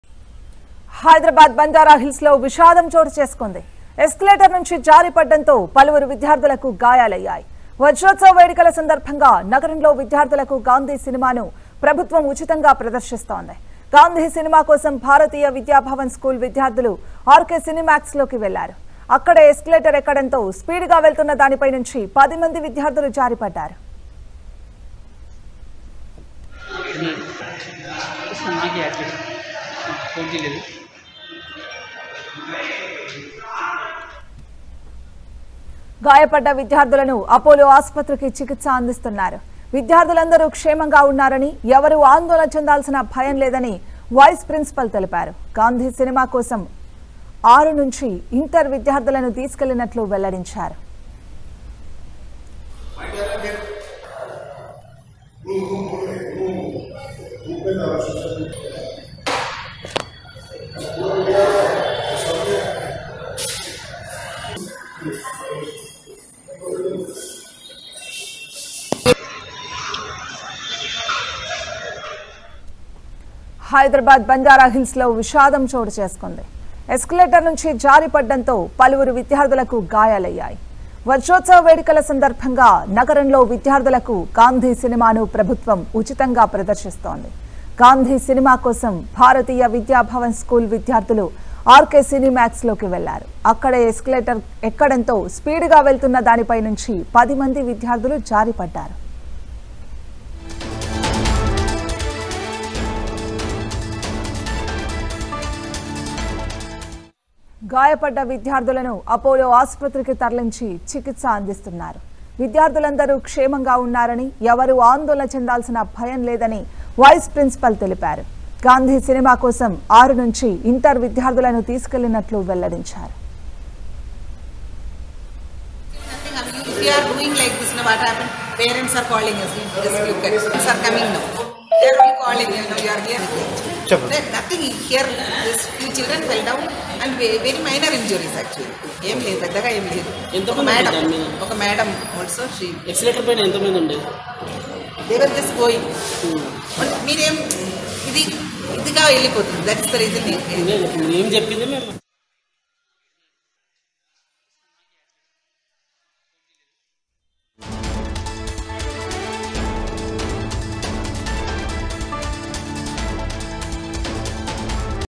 9 Students Injuried After Falling From Moving Escalator At RK Cineplex | TV5 News Digital
ఎస్కేలేటర్ వేగంగా కదలడంతో..!